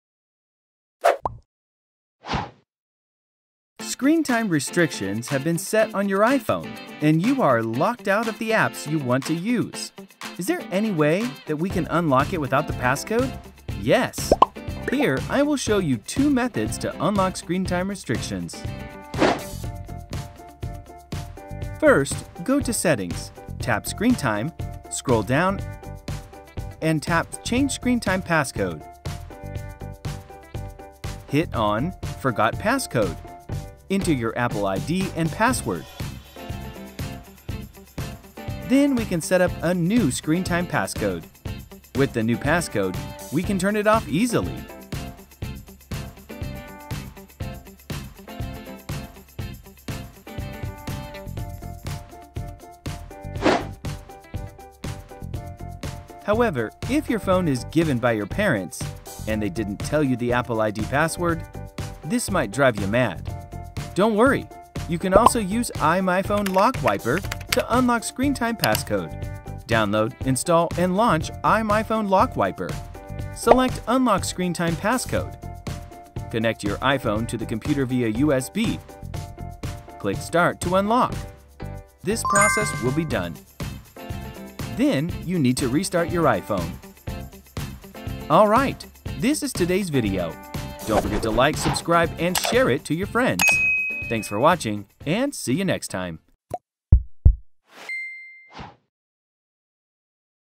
Unlock Screen Time Restrictions on iPhone
This video will show you how to unlock Screen Time Restrictions on iPhone without password.